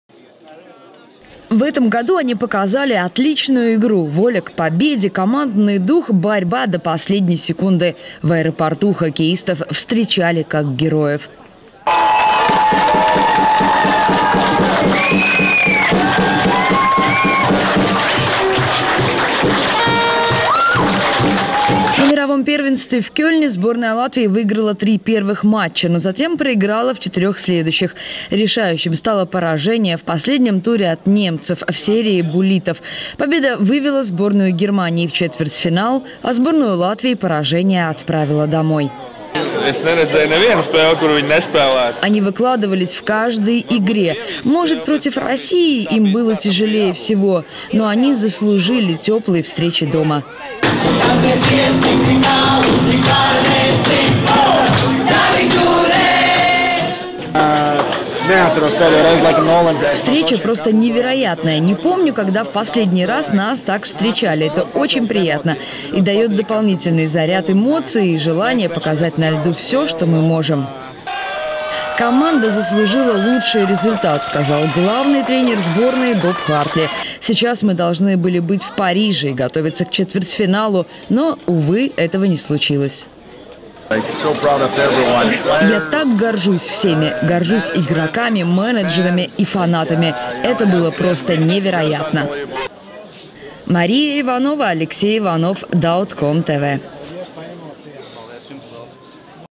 Сборную Латвии по хоккею в аэропорту встречали десятки болельщиков
Латвийская сборная по хоккею вернулась с чемпионата мира в Германии. По итогам турнира наша команда заняла 10 место. В Рижском аэропорту хоккеистов и тренера встречали десятки болельщиков – репортаж Марии Ивановой.